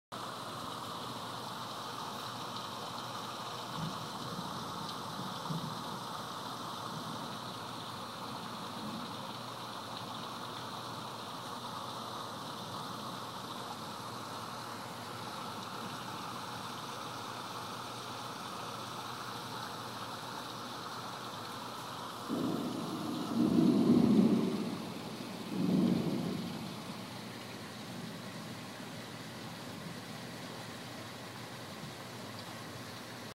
A normal afternoon on the Gulf Coast. I blame BP ;)
Just a typical afternoon T Storm on the gulf coast.  The only thing missing is the hail..